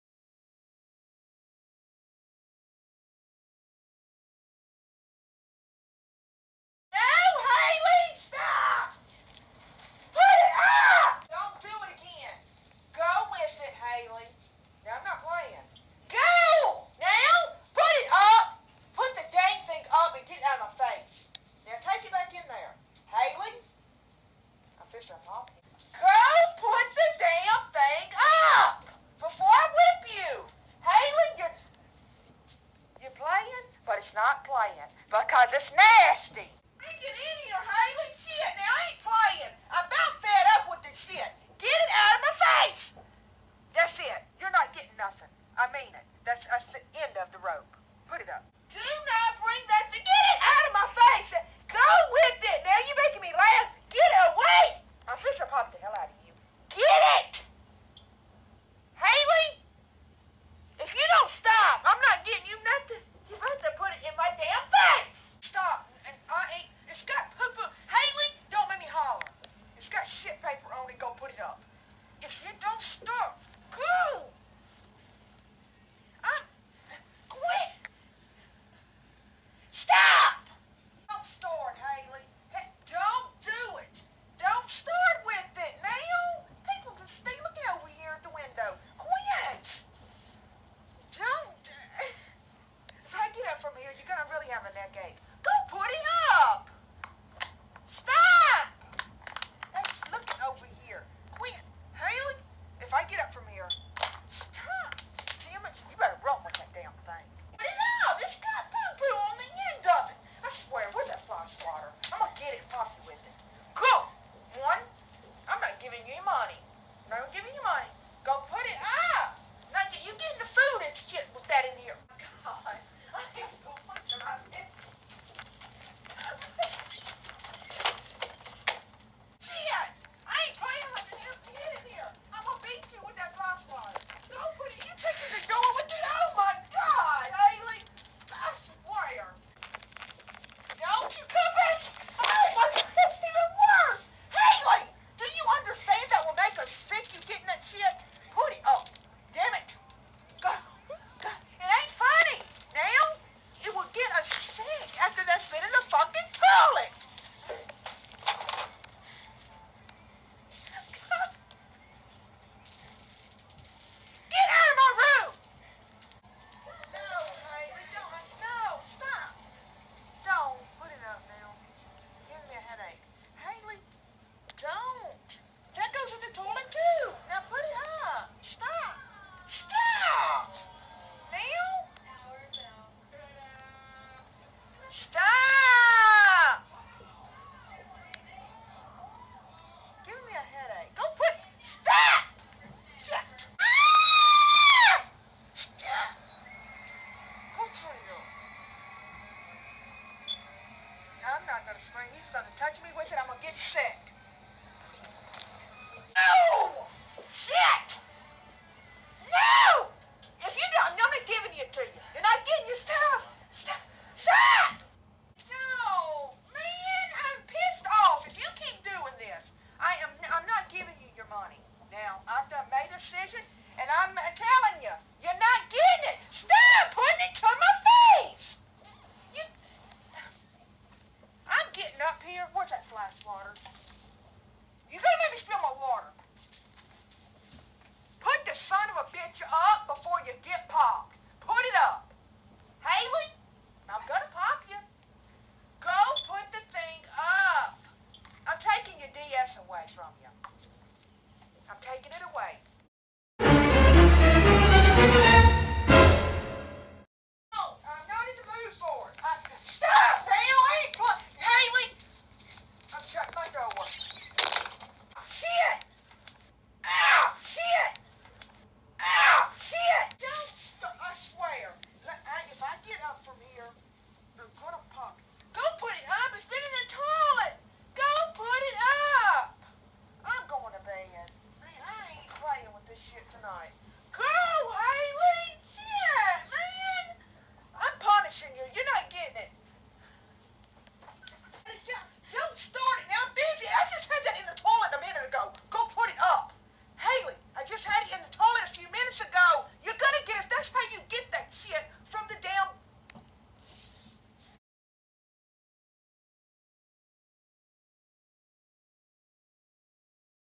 PLUNGER MADNESS
This is a video I put together of me scaring and pranking my mom with a plunger. This video is really funny to me, I couldn’t stop laughing while editing this. This was all filmed a few years ago off of my 3Ds.